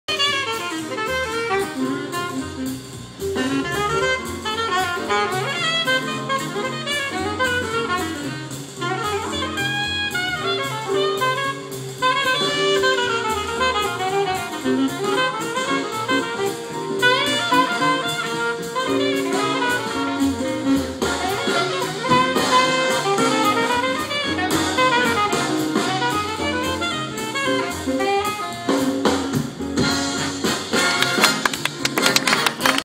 Sydney All Star Big Band at Manly Jazz Festival 2010
Andrew Robertson on alto sax doing a solo.